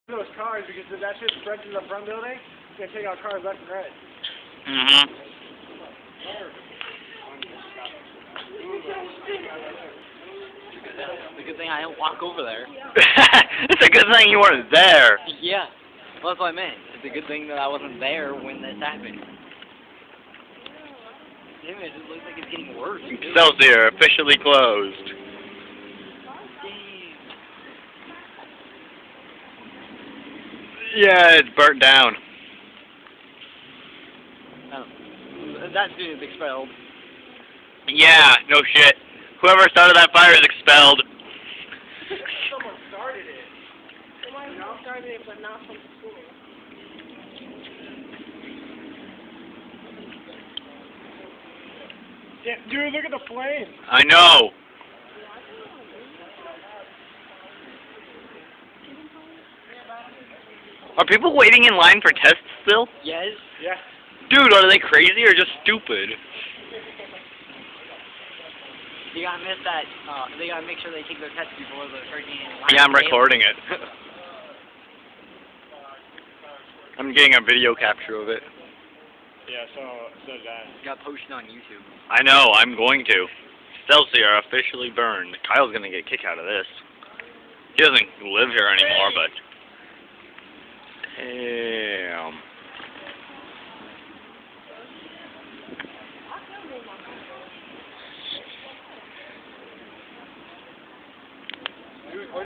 Excelsior Burning
Took this video when I could have sworn Excelsior was a goner. It was quite a cool event. Tons of students started running over there.

However, as it turns out the building behind it was burning. It was a controlled burn by the Victorville firesquad.